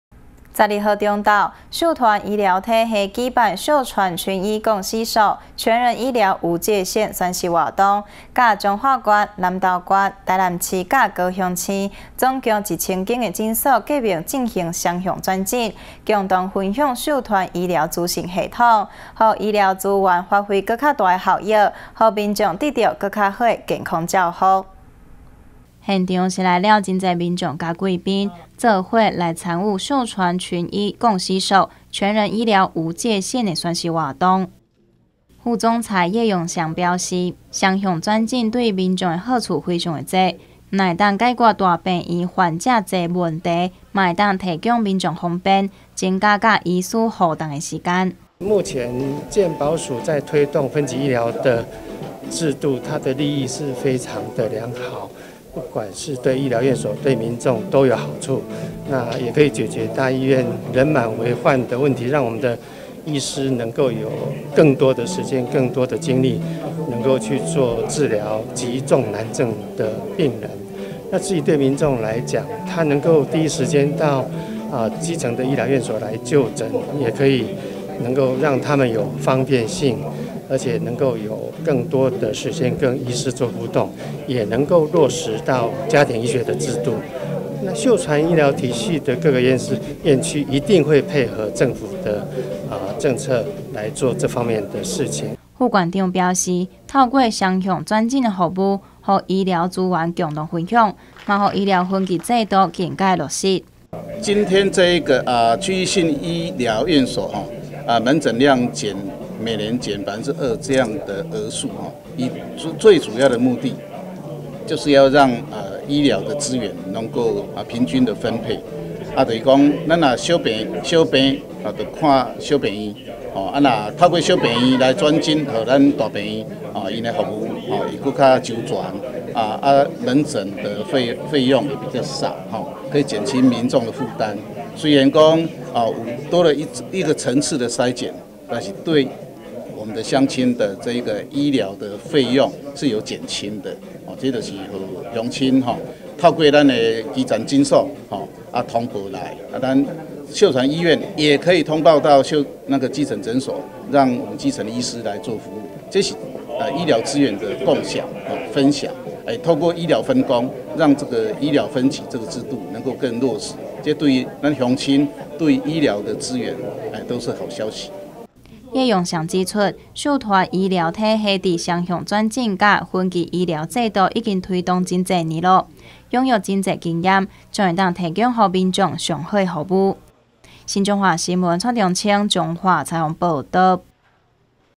新彰化新聞20180712 秀傳與千家診所結盟 共享秀傳醫療資訊系統
12號中午，秀傳醫療體系舉辦「秀傳群醫共攜手、全人醫療無界限」宣示活動，與彰化縣、南投縣、台南市及高雄市共1000家診所結盟進行雙向轉診，共享秀傳的醫療資訊系統，使醫療資源發揮更大效益，讓民眾獲得全人的健康照護。